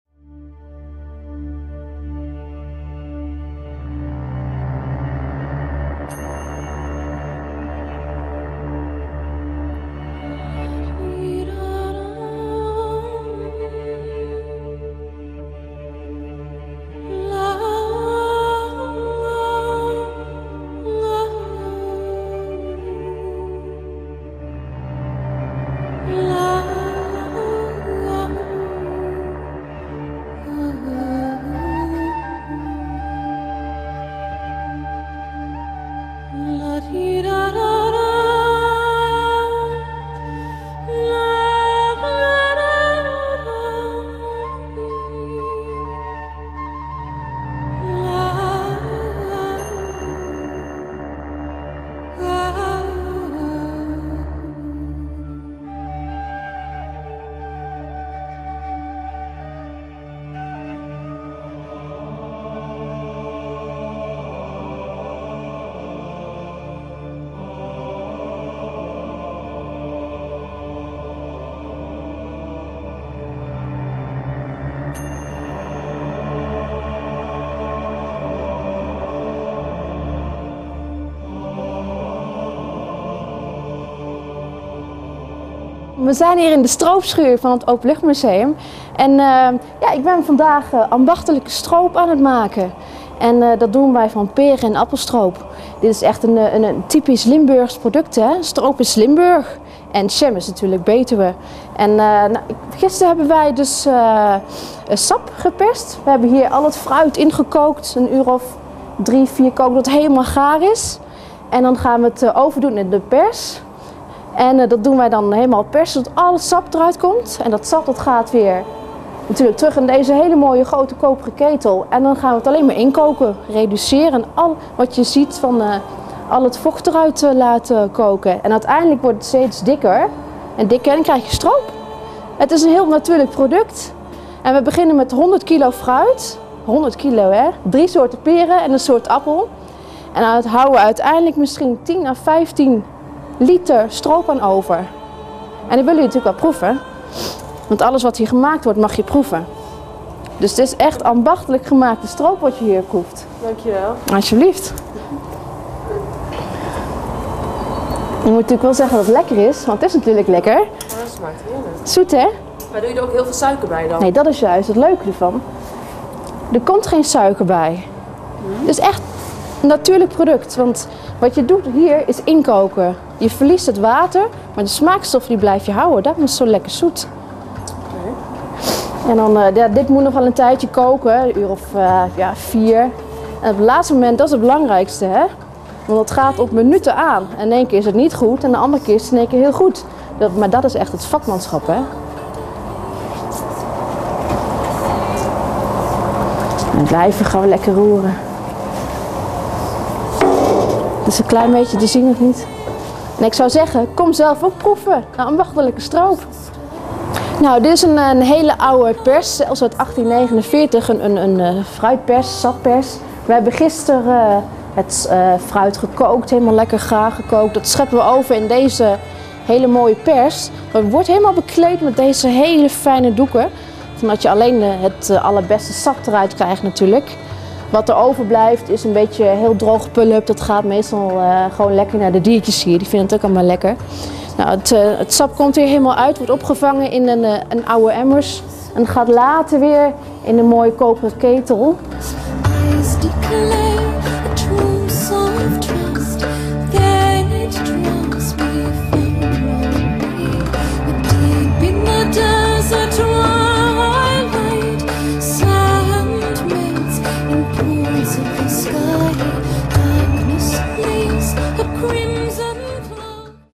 Openluchtmuseum -- de Stroopkokerij
Tijdens de Winteropenstelling van het Openluchtmuseum is ook de Stroopkokerij te bezoeken.